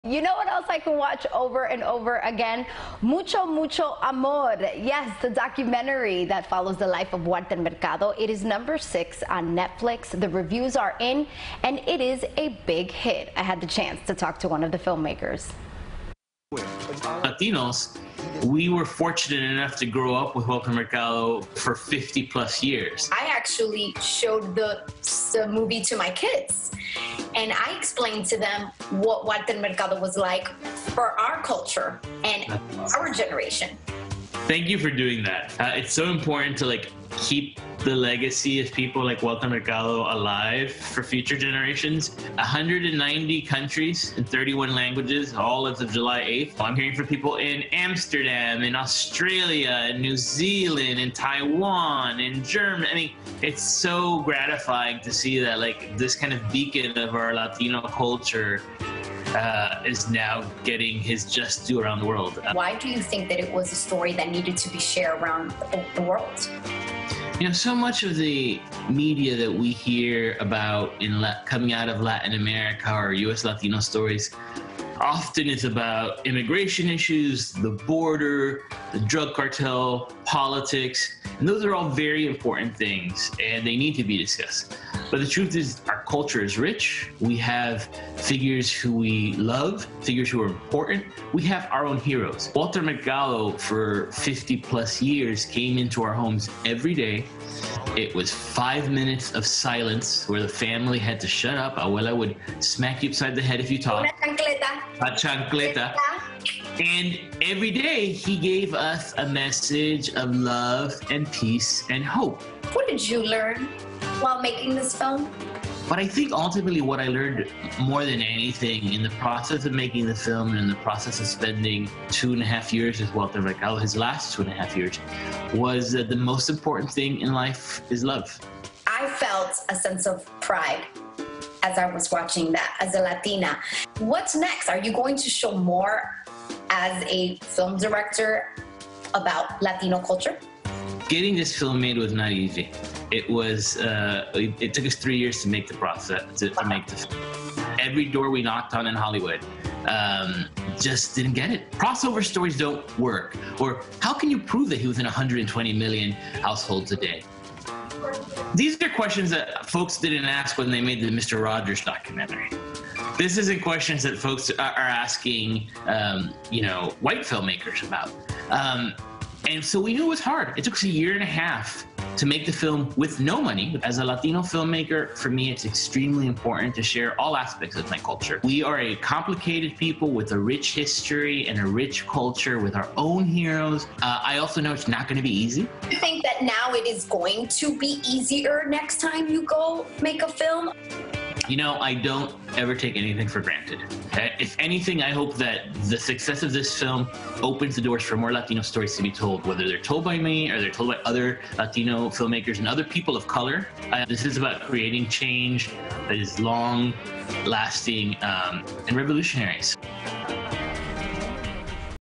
Filmmaker Talks Trending Walter Mercado Documentary
Kareem Tabsch talks about how the man who was a staple in homes across South Florida and around Latin America is getting a new look across the globe